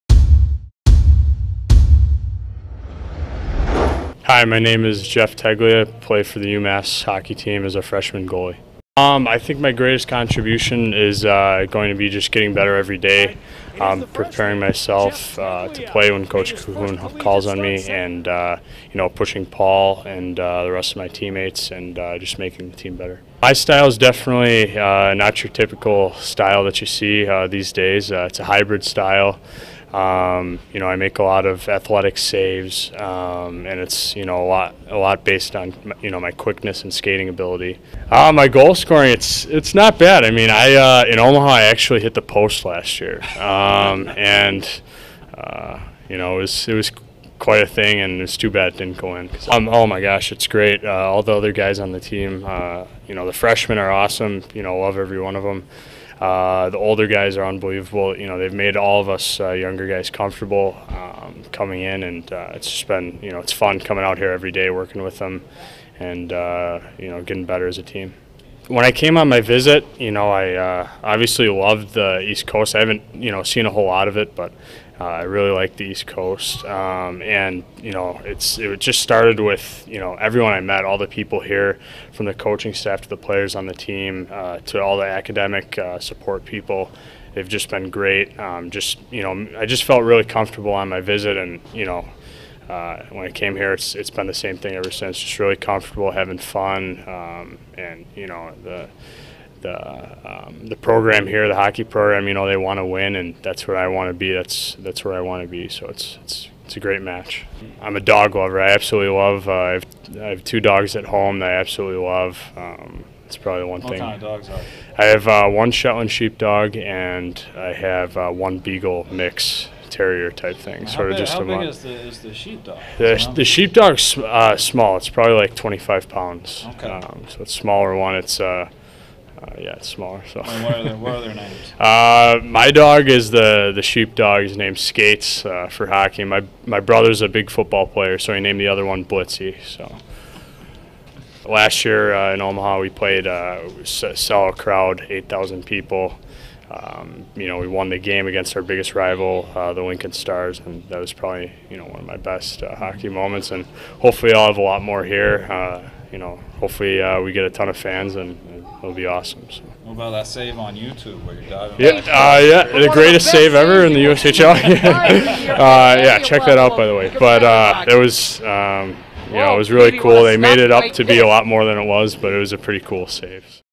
UMass Hockey: Meet Jeff Teglia
In this episode of the Meet The Minutemen series, UMass All-Access takes a few minutes to introduce you to freshman goalie Jeff Teglia.